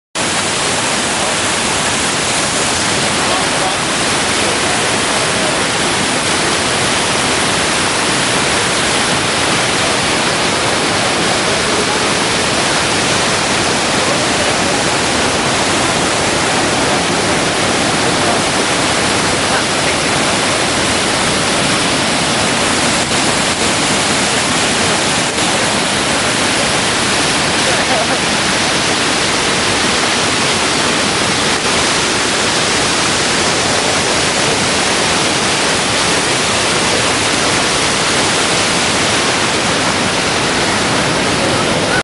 MANAVGAT WATERFALL - TURKEY - AUGUST 2010
MANAVGAT WATERFALL IN TURKEY IN SUMMER 2010 (AUGUST)
MANAVGAT BETWEEN ANTALYA AND ALANYA IN TURKEY.